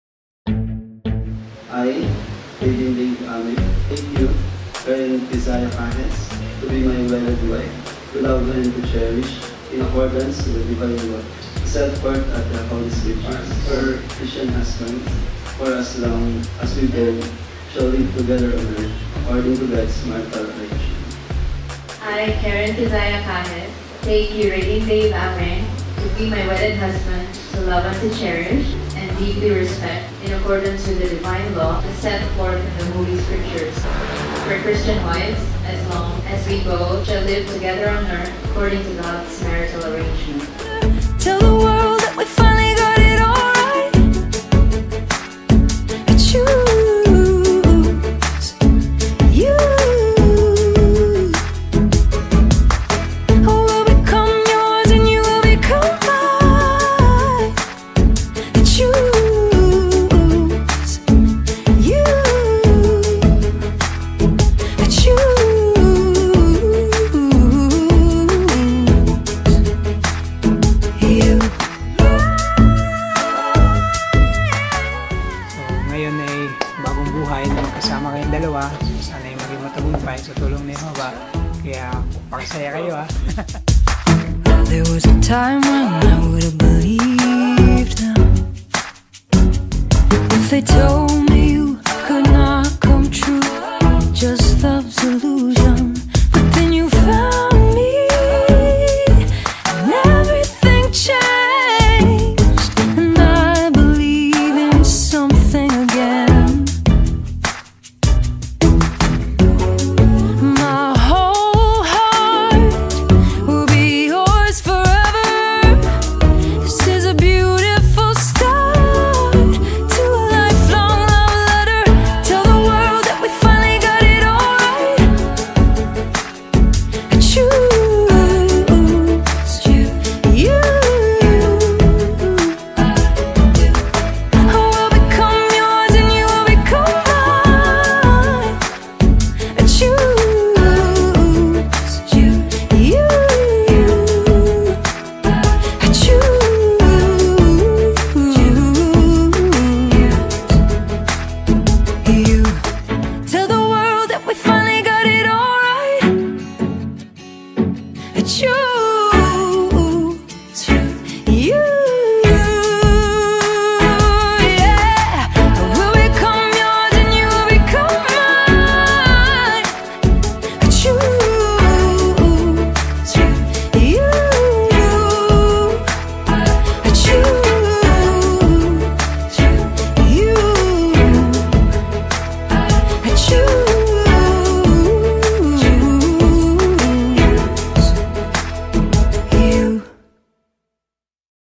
The Wedding of Dave & Kaye "Trailer"
02-19-16 @ Kingdom Hall of Jehovah's Witnesses Talisay City, Cebu